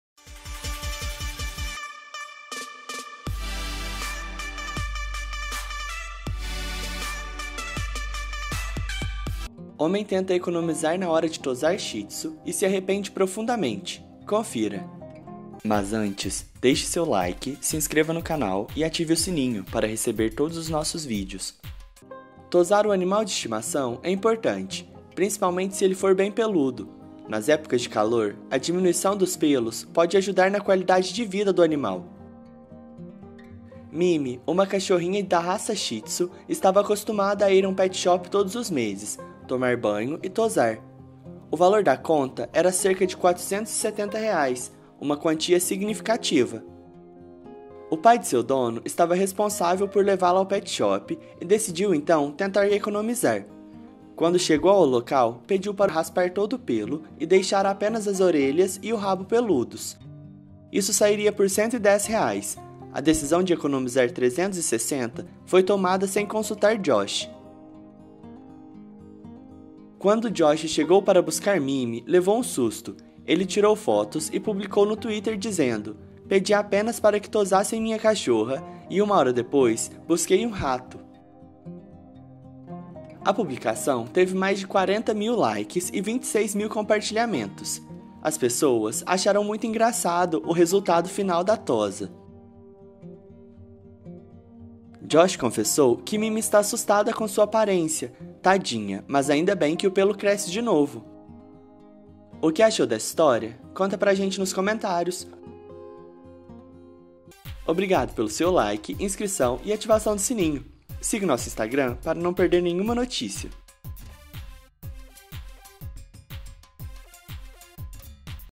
Homem tenta economizar na hora de tosar Shi Tzu e se assusta quando vê o resultado final
Para ver mais notícias interessantes como essa não deixe de acompanhar nosso Instagram